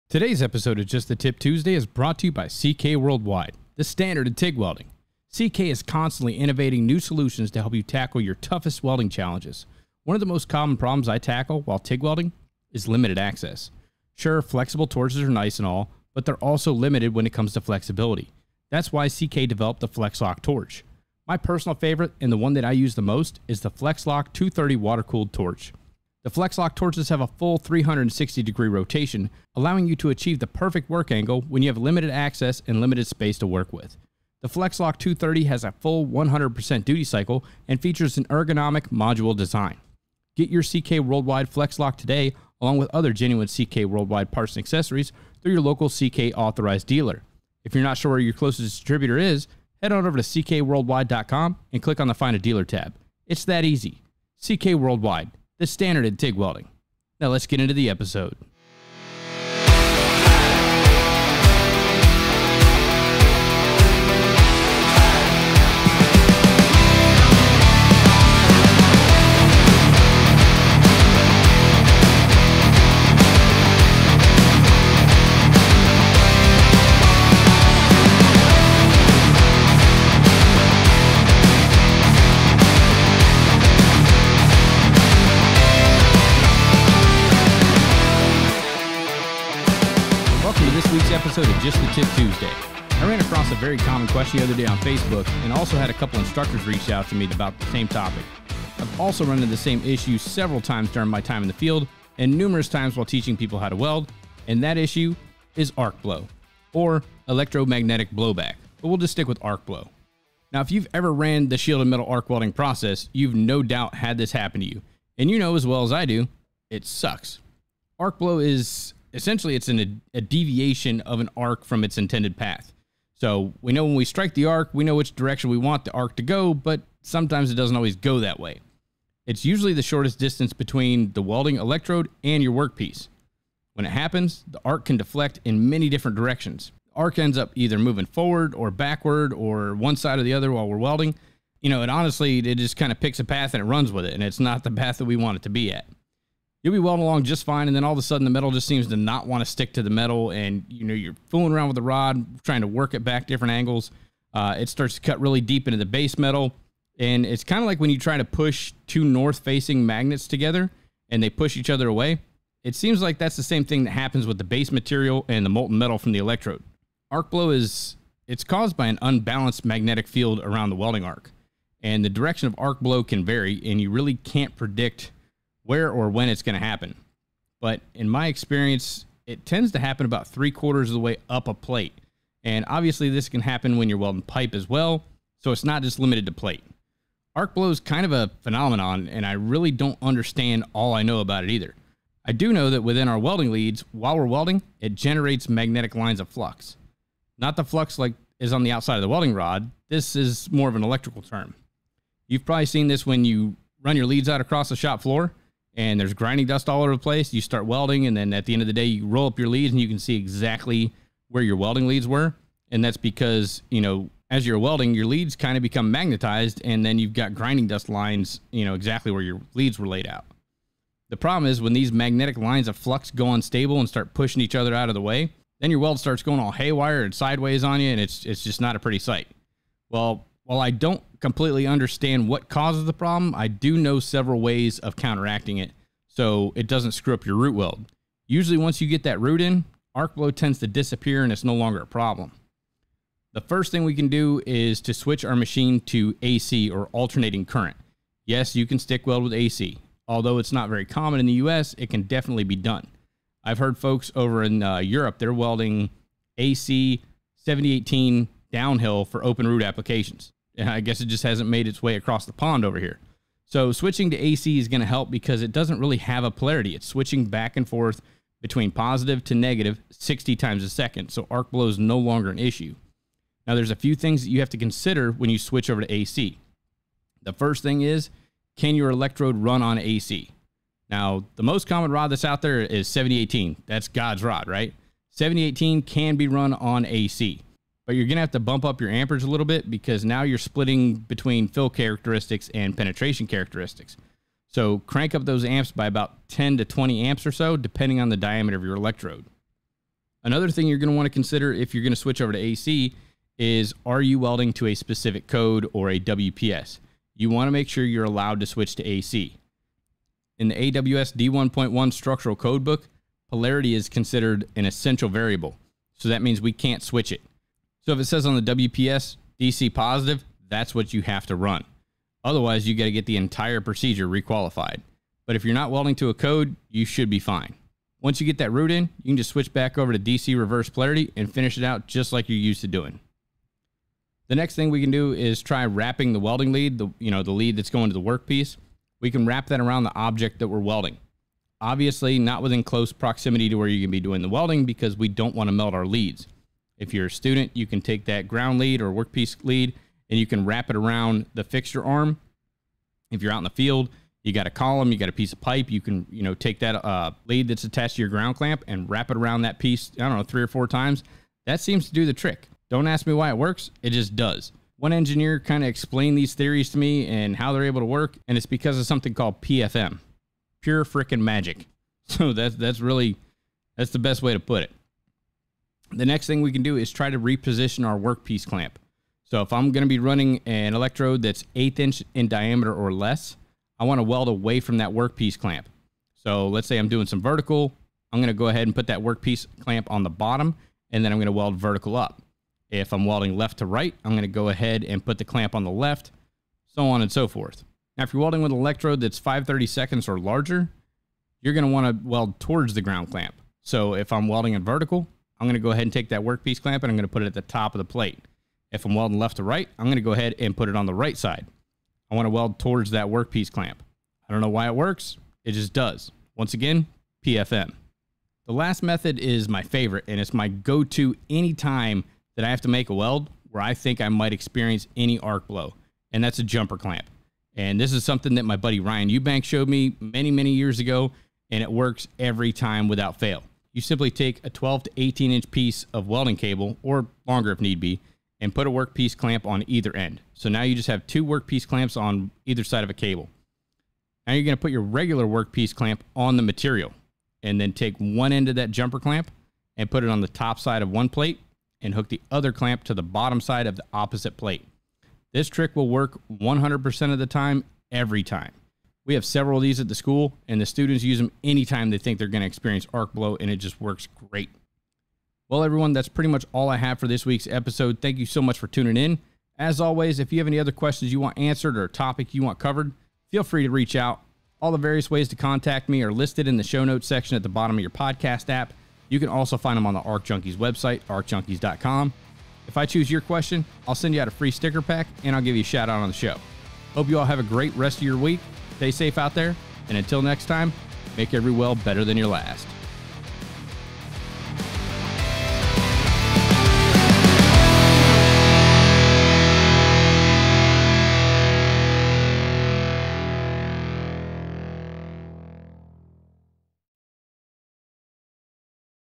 Just The Tip Tuesday Oh No, Arc Blow
In this weeks episode I'm giving you a couple of ways to eliminate a problem many stick welders face on a daily basis and that is Arc Blow.

 Supporter of the Show CK Worldwide